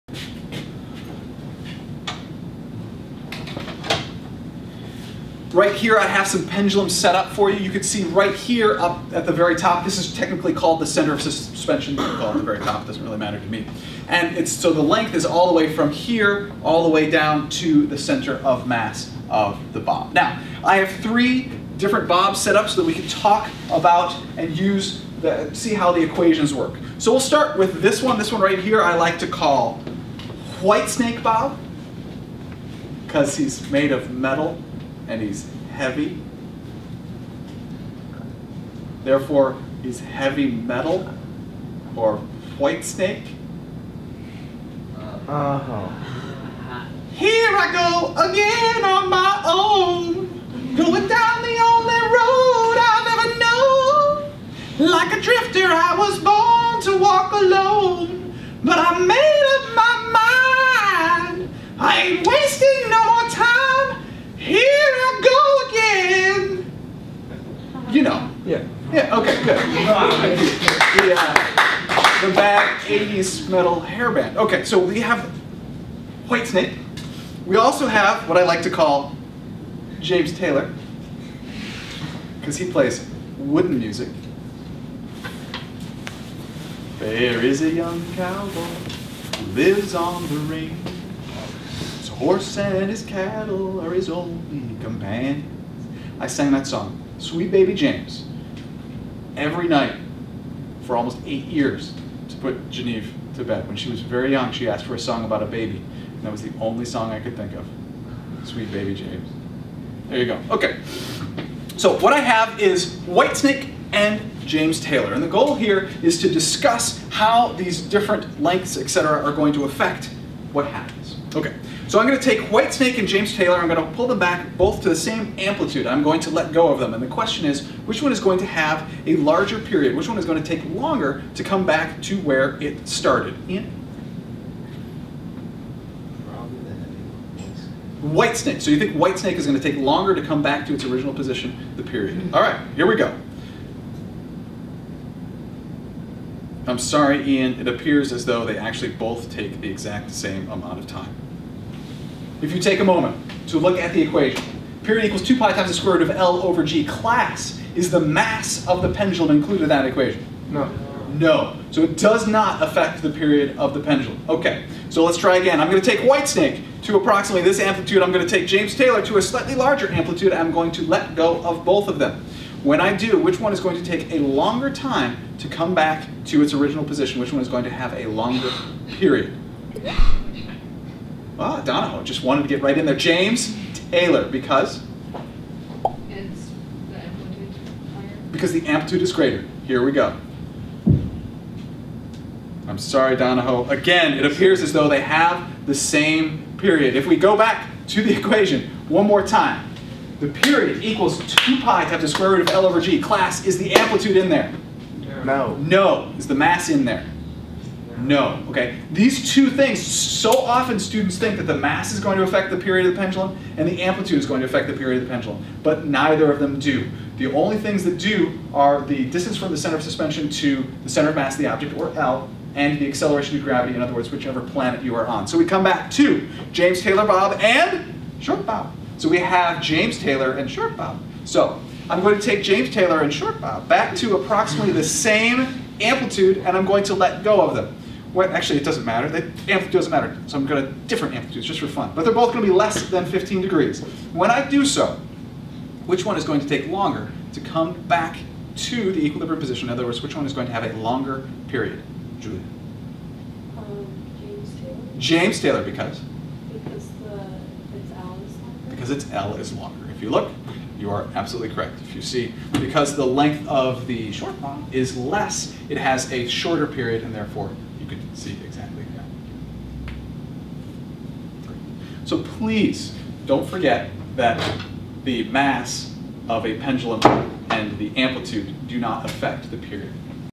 Demonstrations of the Period of a Pendulum - Algebra Based Physics
Algebra Based Physics: Demonstrations of the Period of a Pendulum. has Lecture Notes, Groupings and Sequencing of my lecture videos. Yes, I changed websites.